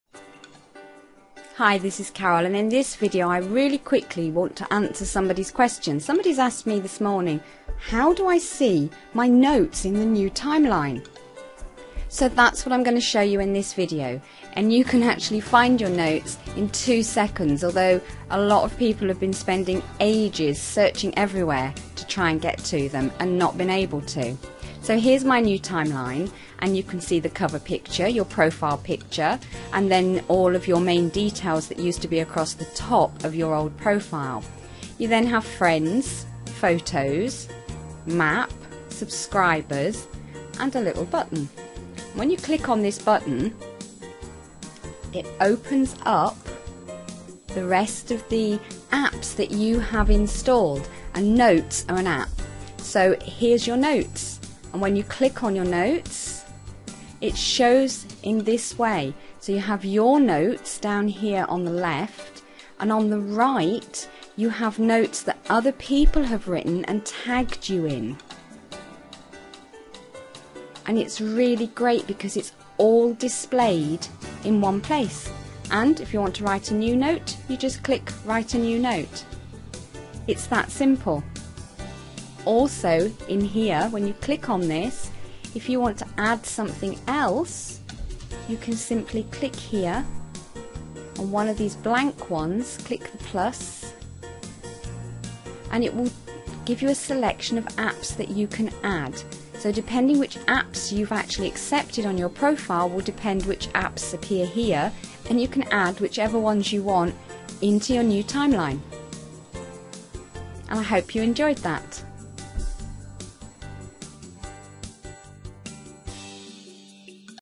Viewing notes and adding apps to your Facebook Timeline
- Carol Dodsley - shows you how to stay up to date and use your new facebook timeline easily and quickly.   In this video Carol - better known as the How2 Girl shows youhow to view your notes, notes you have been tagged in and also how to add apps and functions to your timeline.